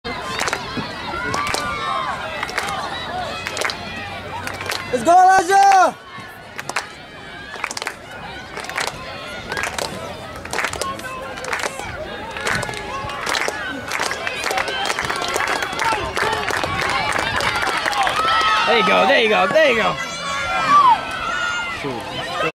Elijah Pitchford Triple Jump Sectionals 2015
Waukesha South High School (Waukesha, WI) senior Elijah Pitchford wins the boys triple jump at the West Allis Hale Sectional meet on 5-29-15 with a 45'2" performance.